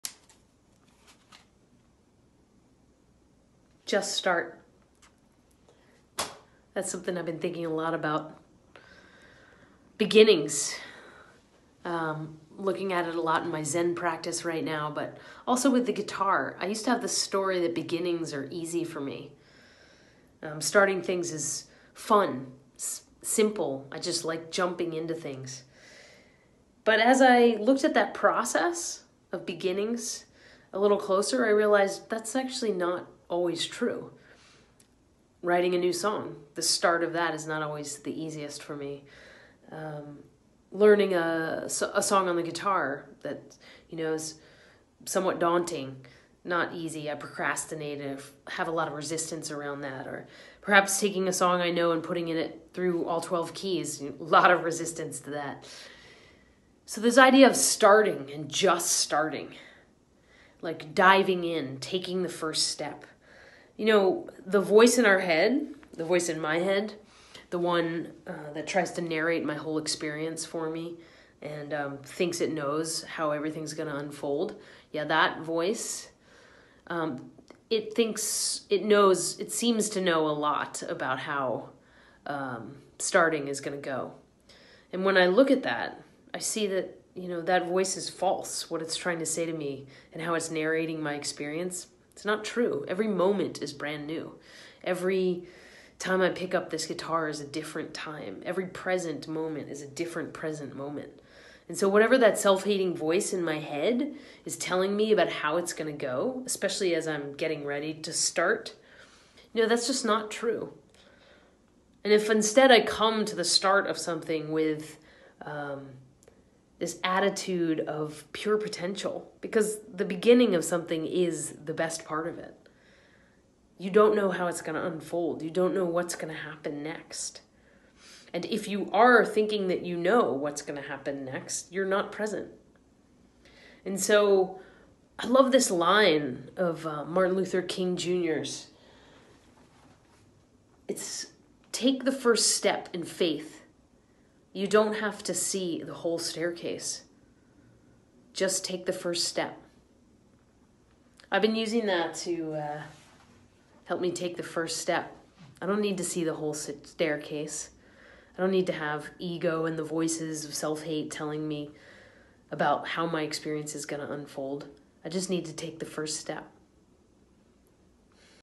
Just start! Take the first step!
Finding it hard to get started? Maybe it’s because you think you know how it’s going to unfold. But do you know? How do you know? What’s your process around beginnings? Around starting?

Starting can be the most exciting part of a project or endeavor.

It’s pure potential! It’s an incredible time to be present.

But you can’t be present or full of energy for the starting when the voices of self hate are in the way.

You know the ones - they’re trying to tell you how it’s all going to go down.

Don’t believe them. Just start! Take the first step!